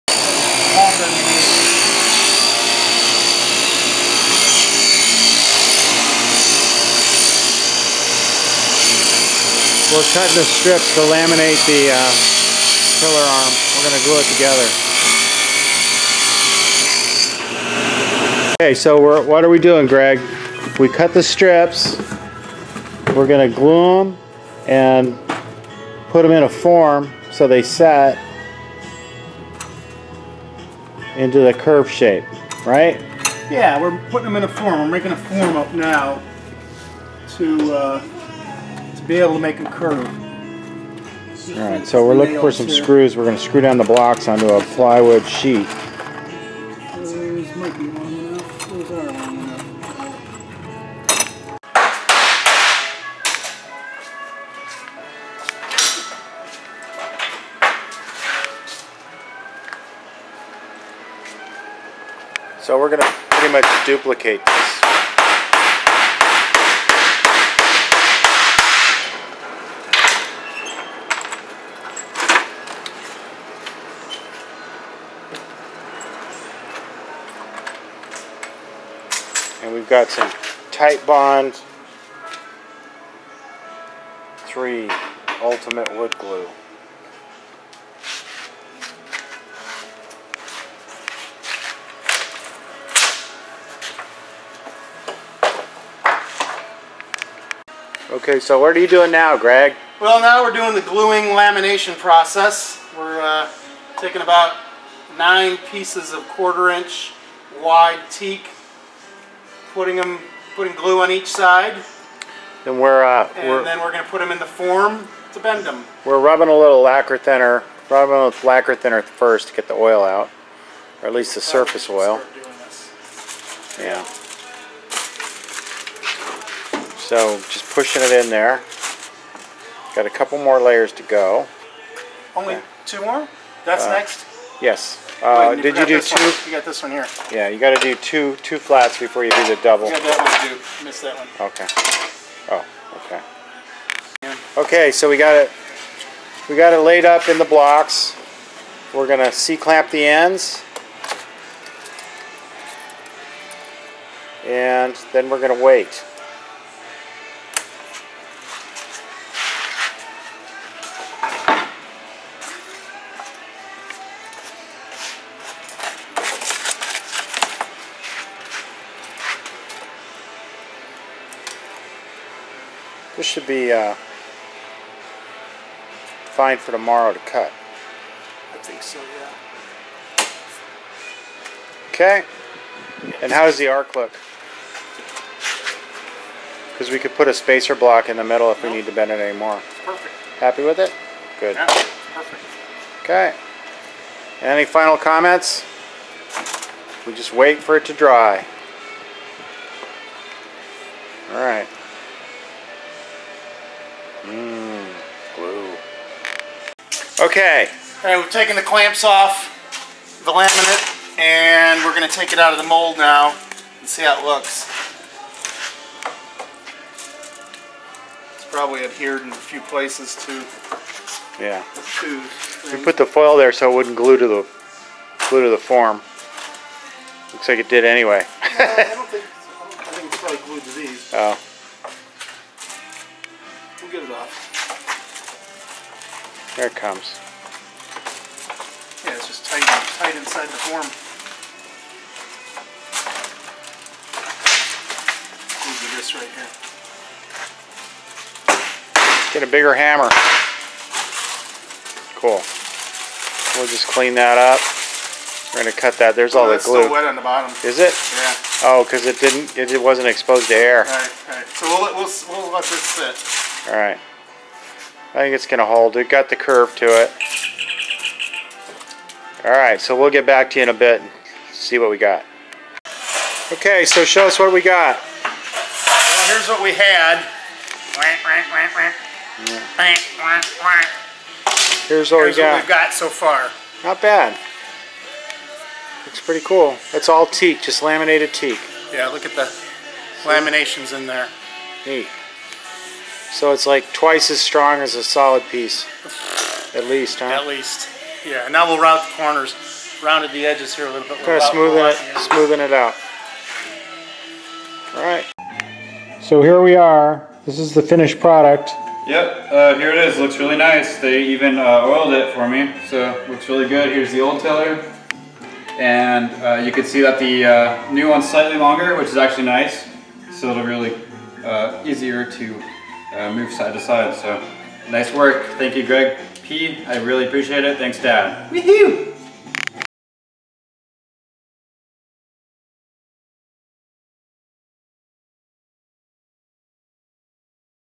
How to Make a Tiller *Bonus Episode!*
Learn how to make a tiller in this "dad edition" of Drew's Cruise featuring my dad and his friend Greg P.